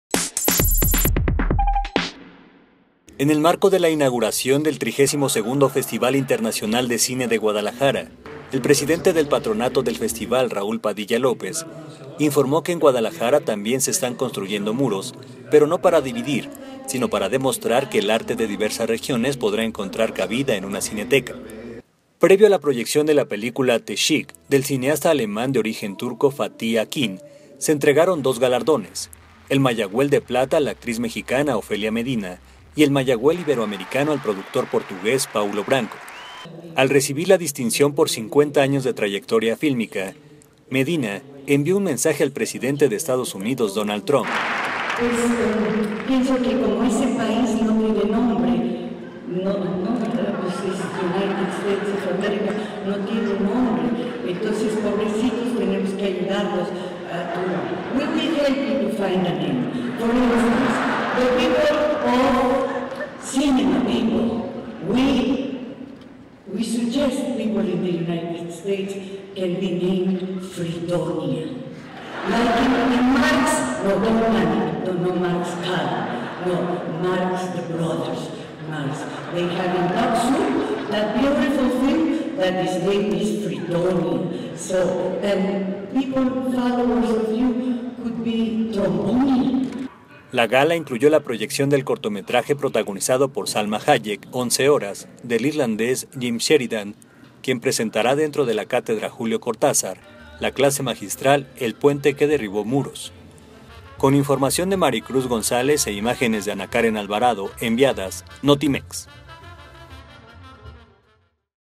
Ofelia Medina y Paulo Branco son reconocidos en el FICG
El presidente del Patronato del festival informó que en Guadalajara también se están construyendo muros, pero no para dividir sino para demostrar que el arte de diversas regiones podrá encontrar cabida en una Cineteca